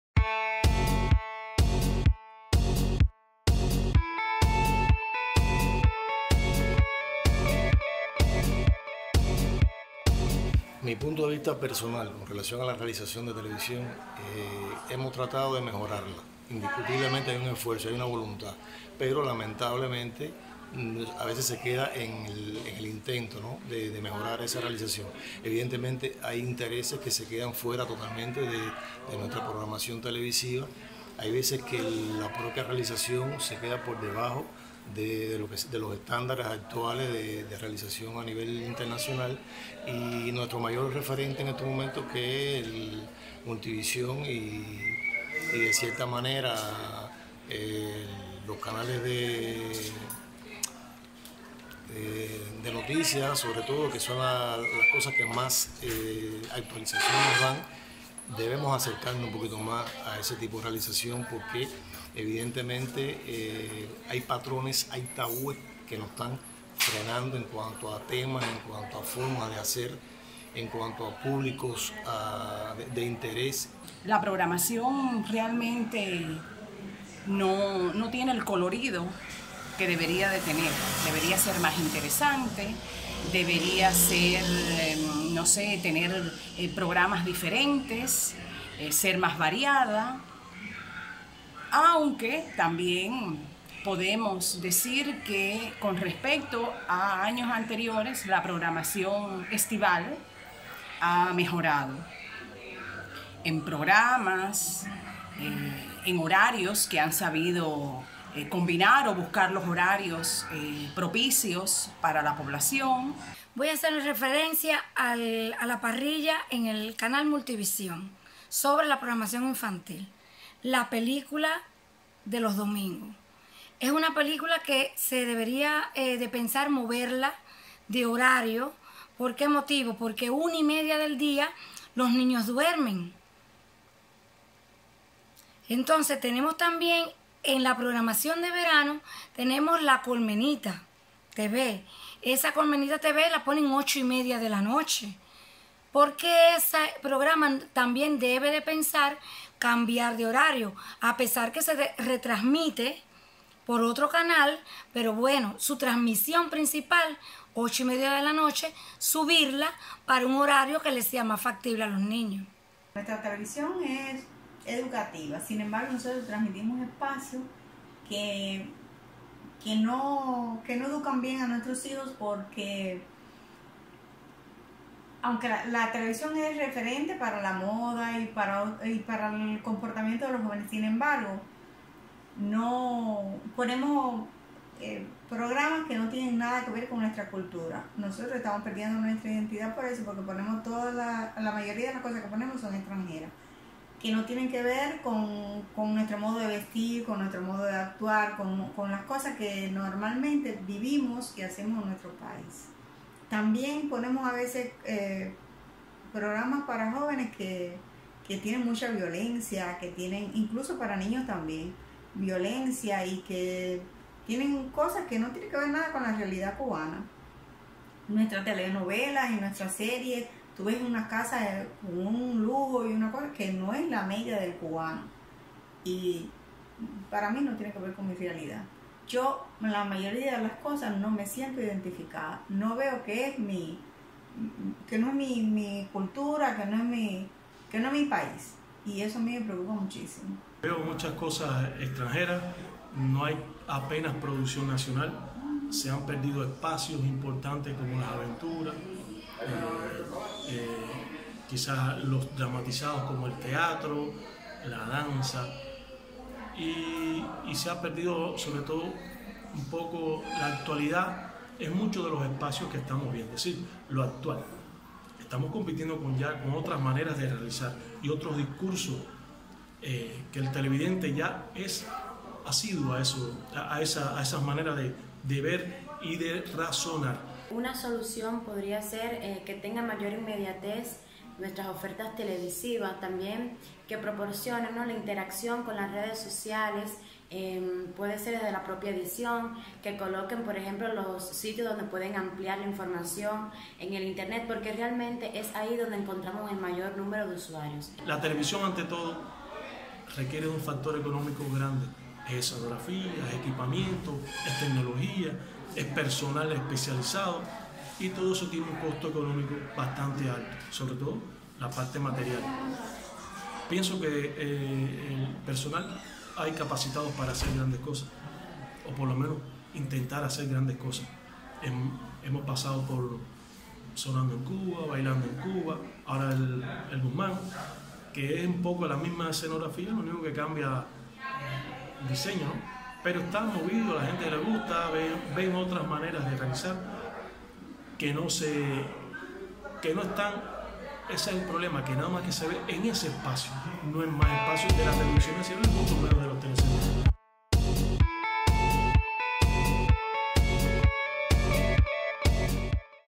Los realizadores de la TV en guantánamo reflexionan sobre la programación televisiva
Como parte del espacio Moviendo los caracoles, los realizadores del Telecentro de Guantánamo Solvisión dejan sus reflexiones sobre la televisión que hacemos.